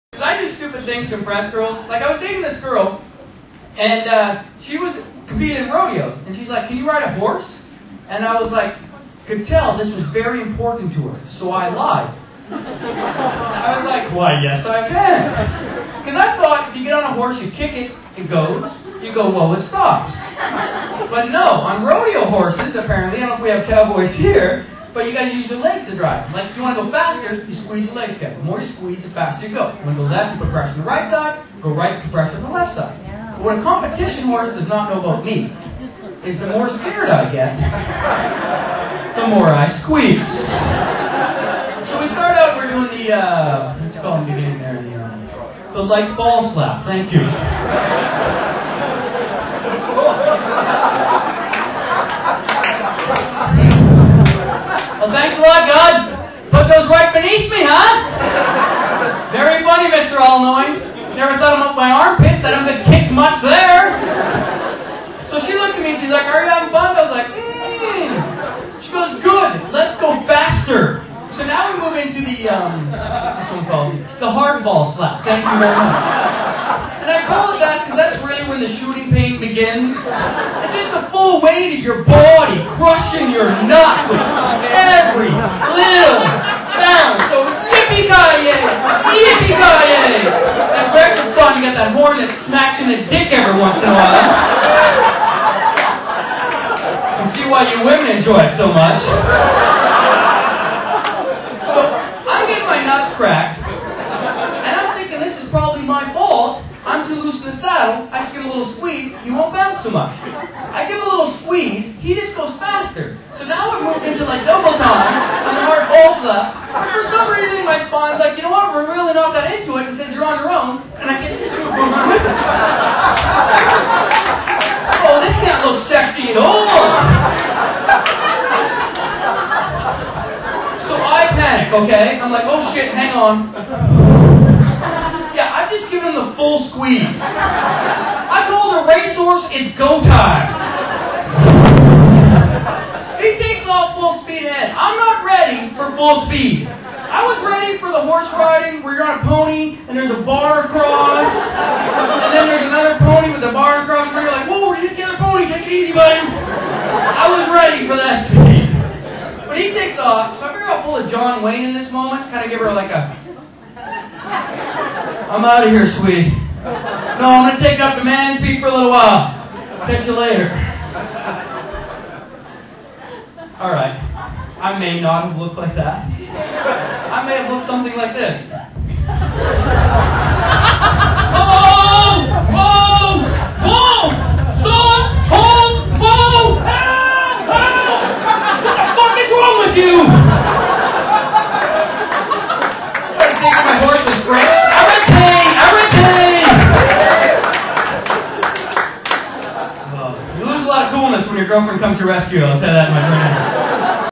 Dan Quinn goes horseback riding
Dan Quinn tells a story about going horseback riding.